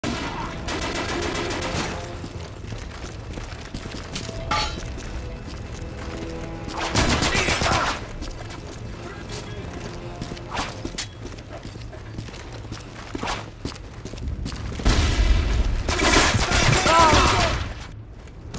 Best & Worst Play Of The Day: (Worst)
Guy's this was an epic fail. Im such A fail at Tactial Knifing.Deuces